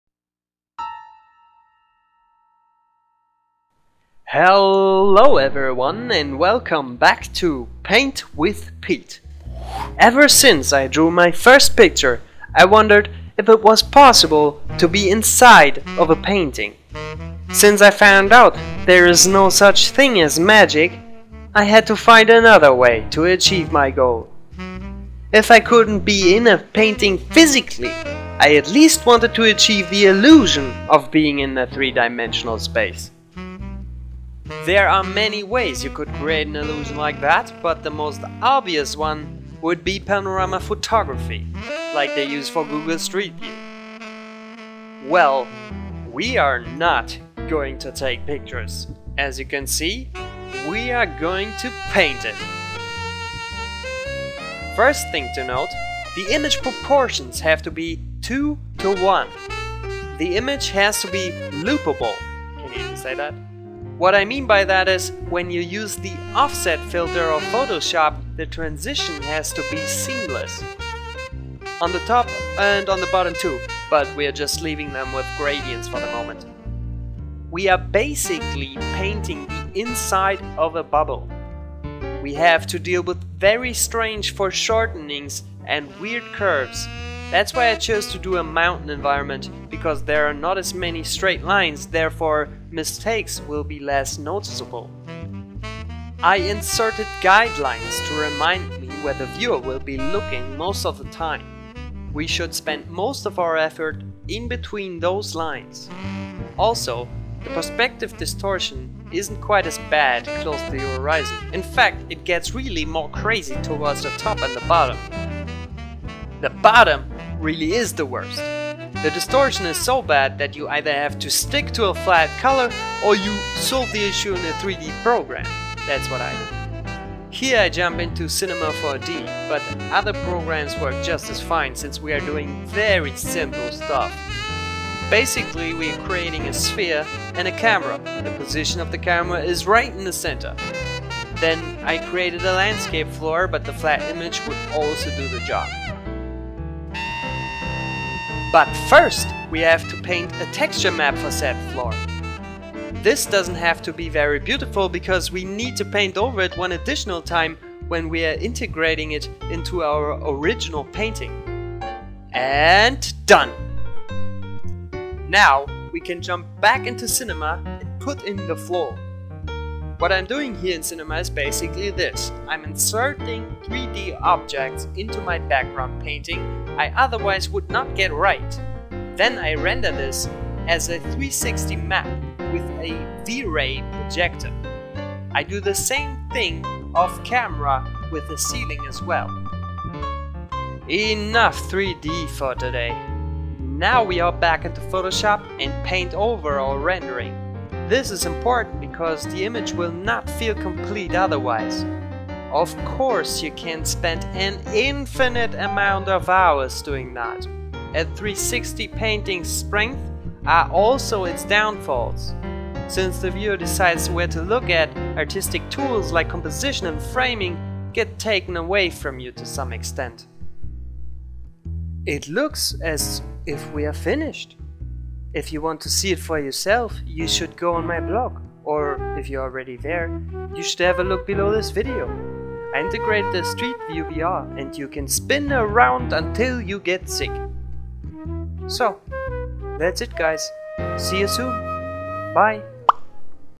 Paint With Pete Episode 03
Ever wanted to be inside a world that you created? Pete can give you the tools to create the illusion to do just that!
This is a spherical panorama created in photoshop, painter and cinema 4d.